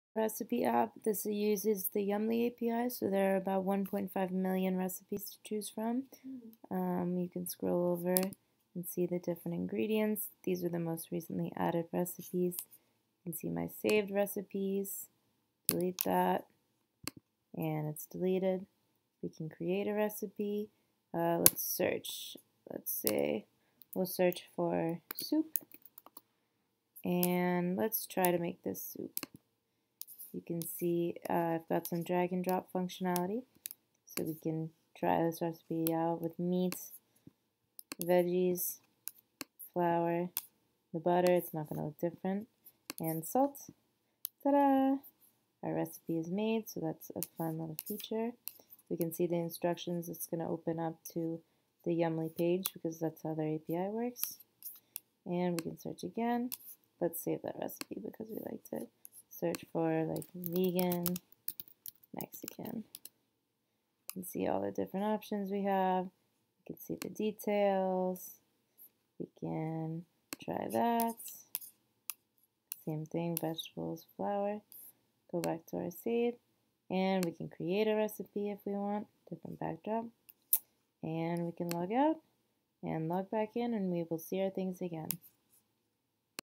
React Redux Recipe App
A Recipe app that I made where users can search the Yummly API of about 1.5 million recipes, save their favorites, create their own, and “try” with a drag/drop game. Had a bit of fun with the styling adding cute animations and the pic and me and ASAP Rocky on every page lol.